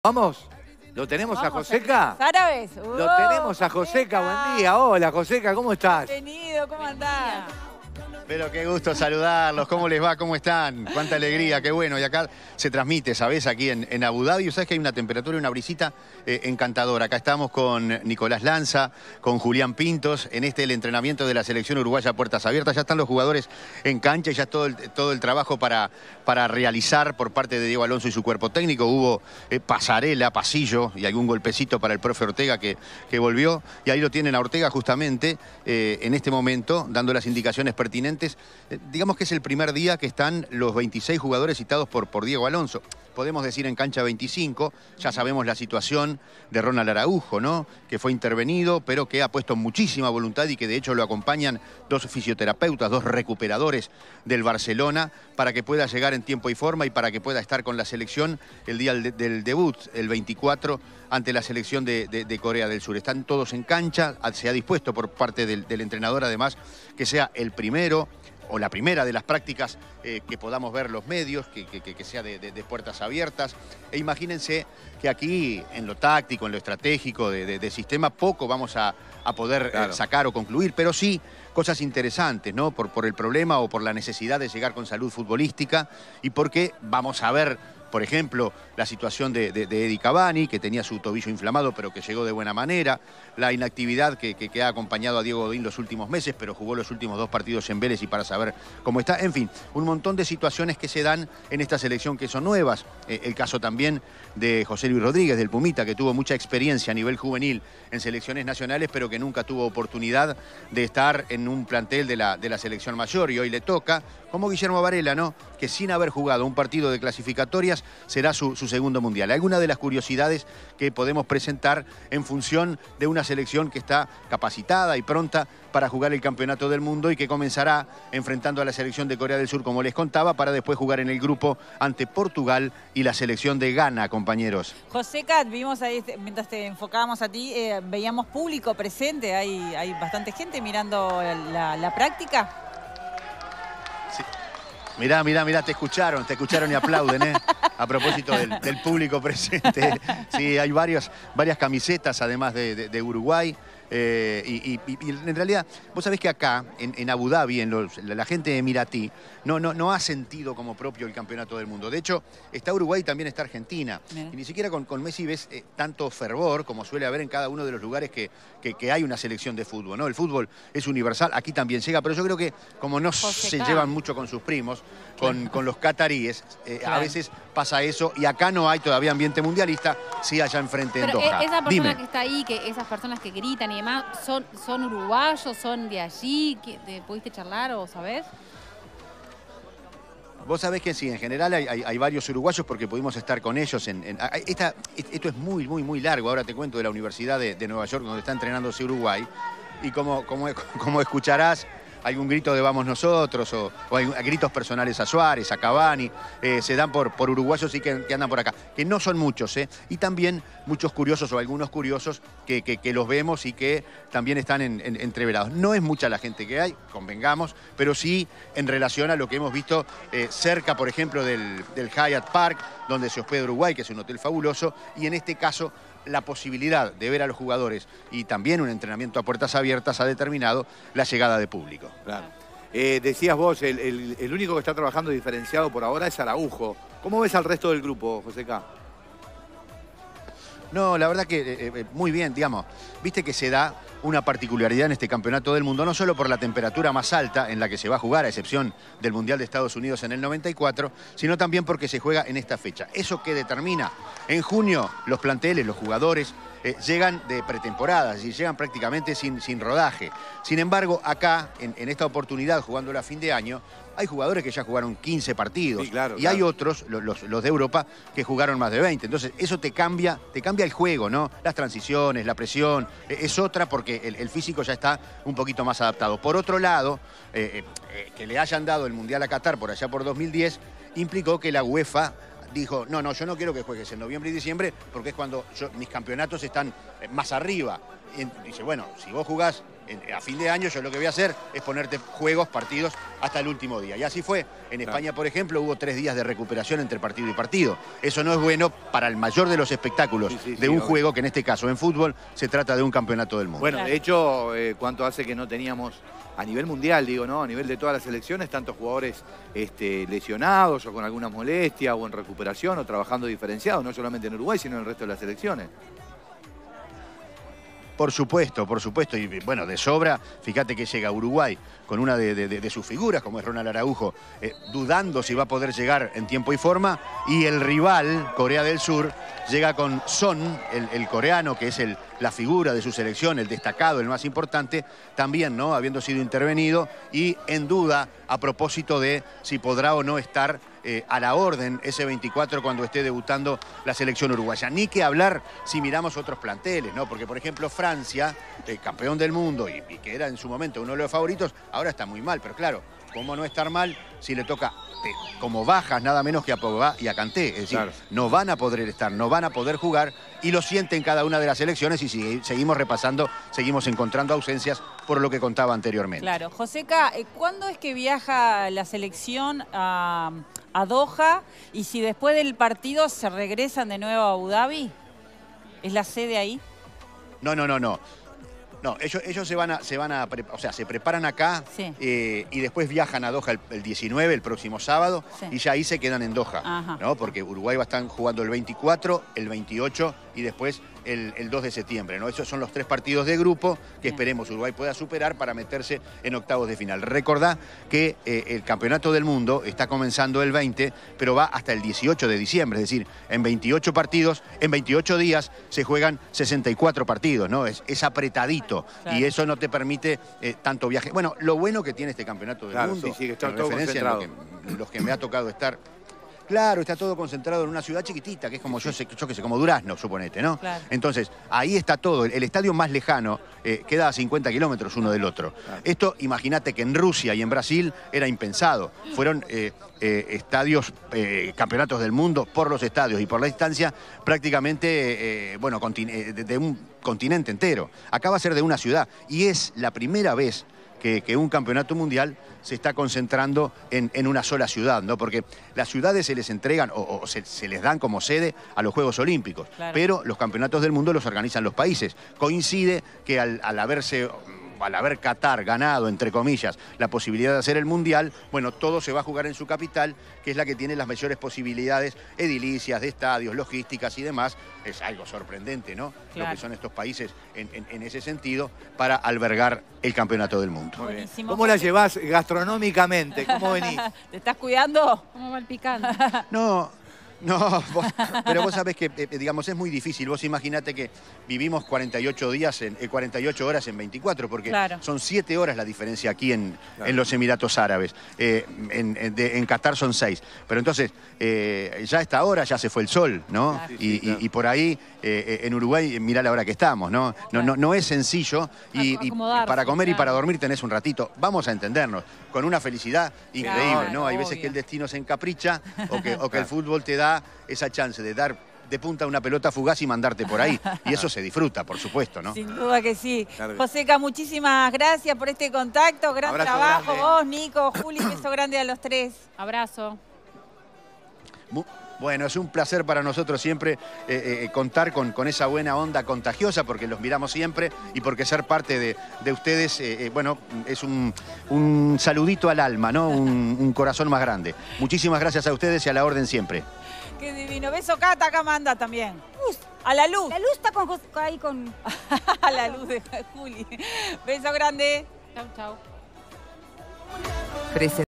Uruguay se prepara para enfrentar a Corea del Sur
Desde el entrenamiento de la Celeste dirigida por Diego Alonso contactamos con José Carlos Álvarez de Ron, quien nos informó sobre el entrenamiento de la Celeste en Abu Dabi. El defensor uruguayo Ronald Araújo trabaja diferenciado del resto del grupo. Uruguay debuta ante la selección asiática el próximo 24 de noviembre.